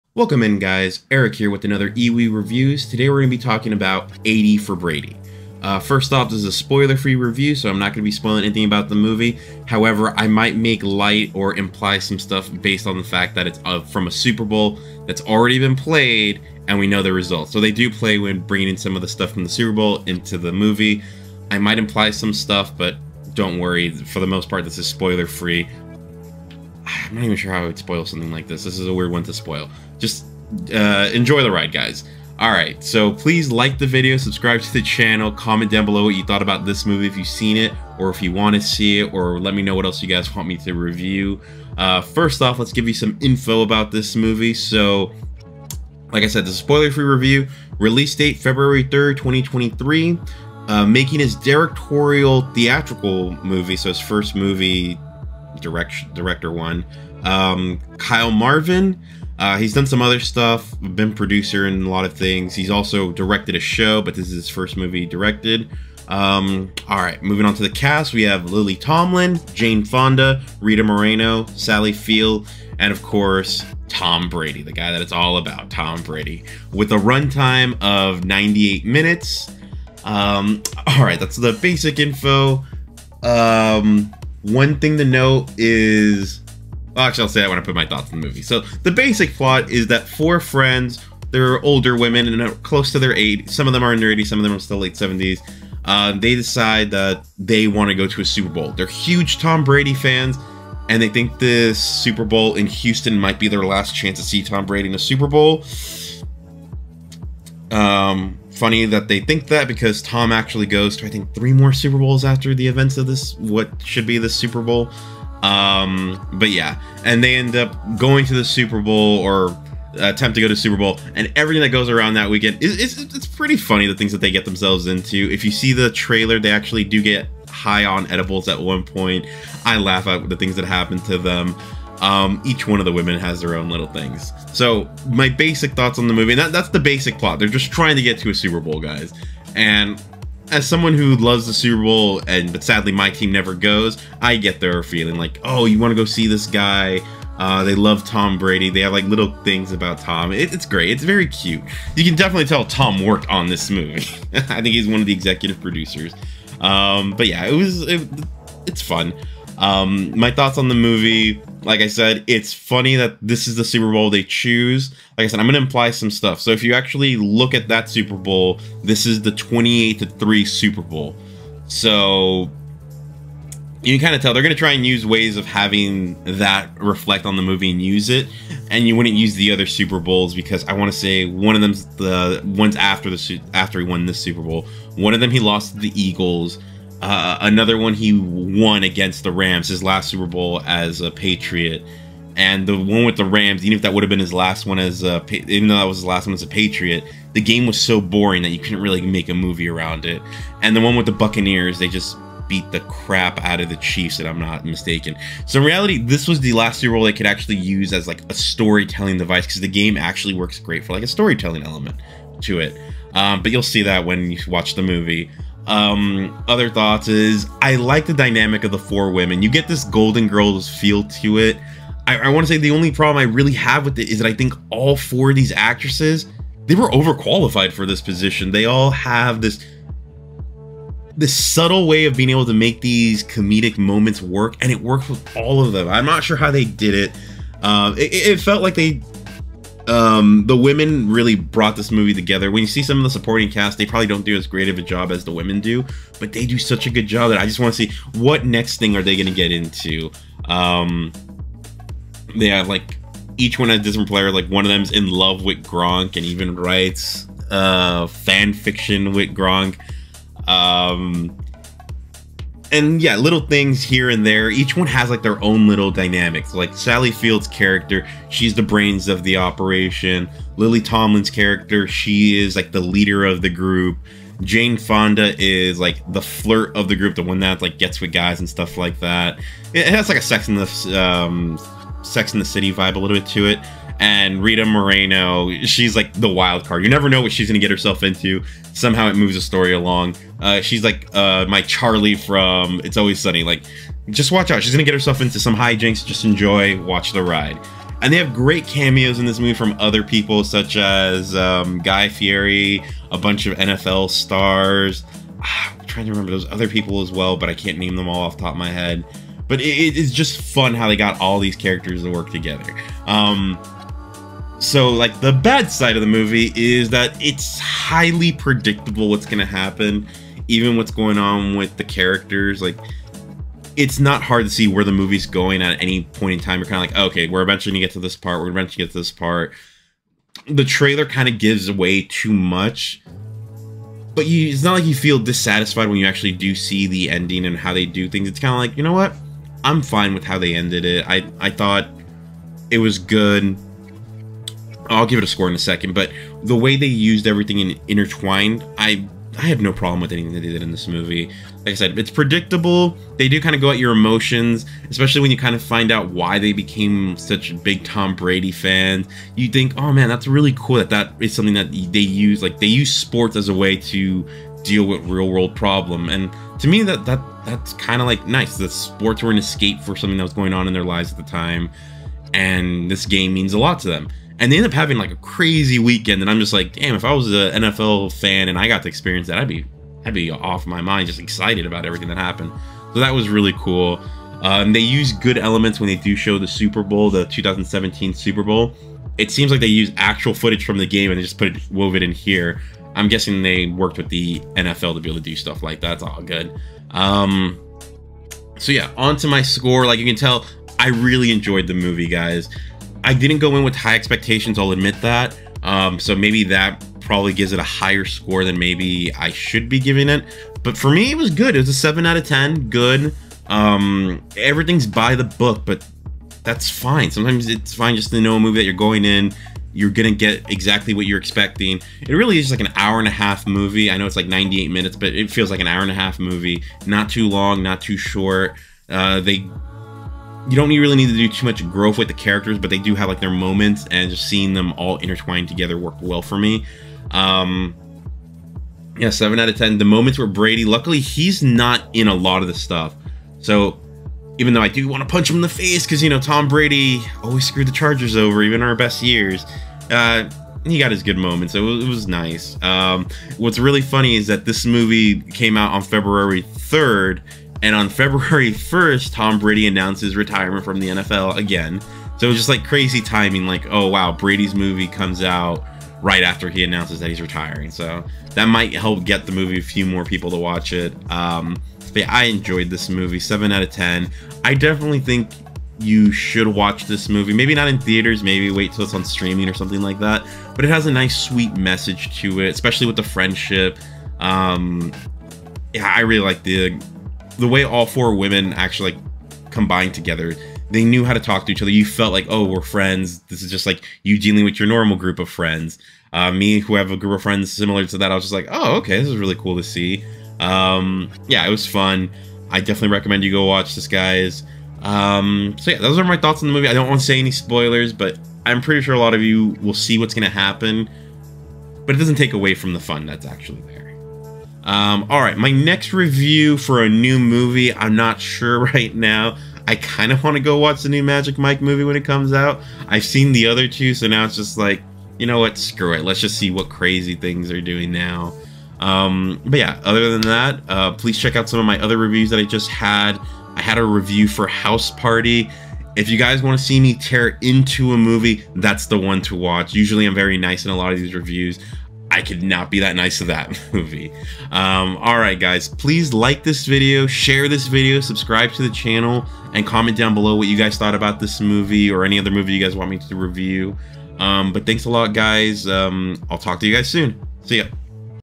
80 For Brady Spoiler Free Review
Eric reviews 80 for Brady. The new movie about for Older Women on a quest to watch Tom Brady compete in Superbowl 51. This is a spoiler free review.  Please check timestamps to jump to which ever section you'd prefer.

Chapters:
0:00 Intro
0:41 Movie Info
2:16 Thoughts on Movie
6:39 Score